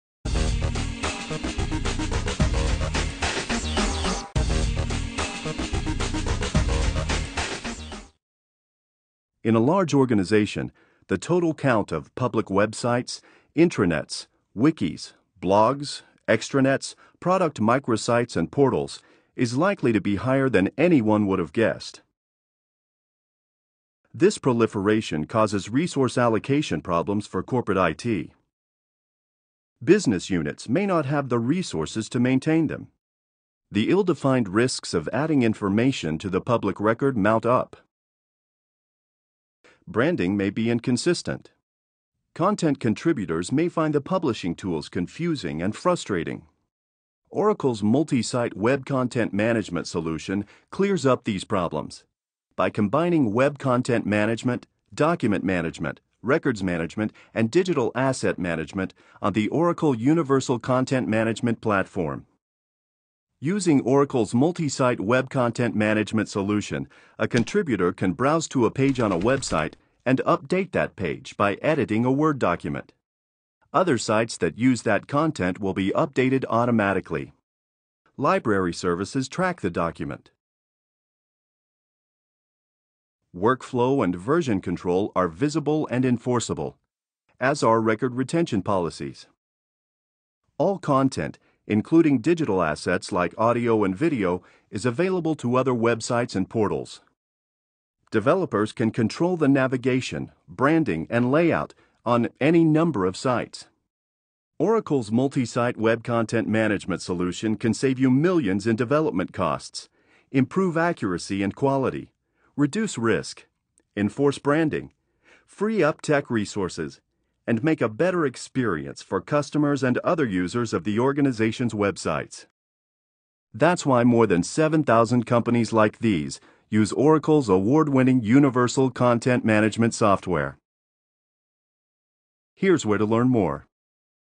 Oracle Web Content Management 2-Minute Explainer Video
Enterprise content management system for companies with large number of product sites, blogs, intranets, partner sites, microsites, etc. 2-Minute Explainer produced by Business Information Graphics